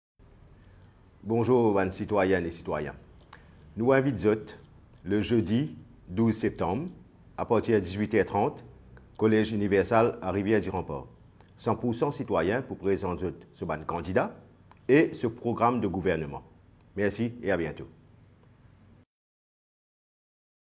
100% Citoyens - 100% CITOYENS invite zot pou ene face-a-face
Jeudi 12 septembre a 18h30, au college Universal a Riviere du Rempart.
Nou pou devoile zot en avant premiere, nou Program de Gouvernement et zot pou kapave discute avec nou ban candidats.
Share a max.